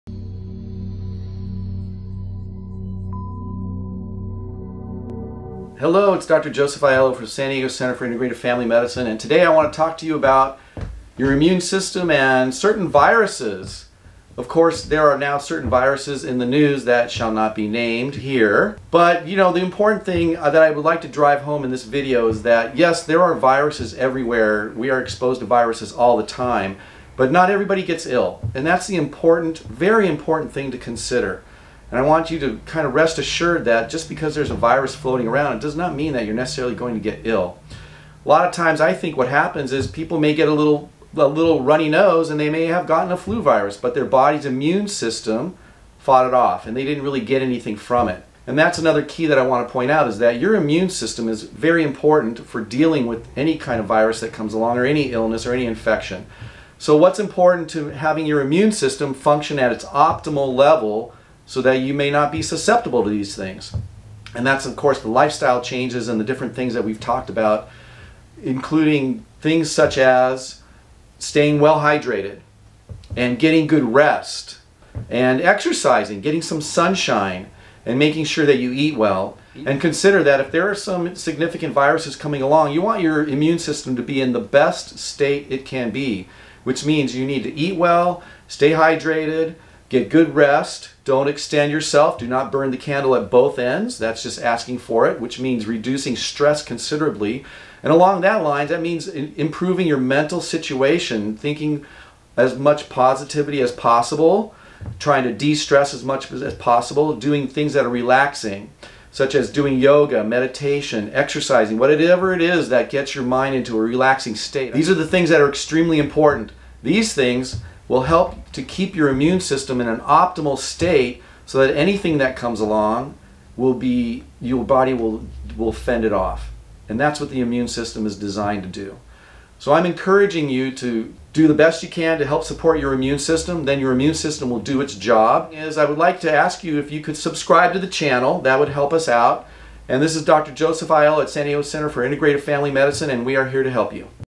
Protecting Your Immune System to Avoid Flu and Illness
With all of the viruses and illnesses featured in the news lately, we shouldn't assume that being exposed means that we will get sick. Bolstering your immune system naturally can protect us and just takes some common sense steps.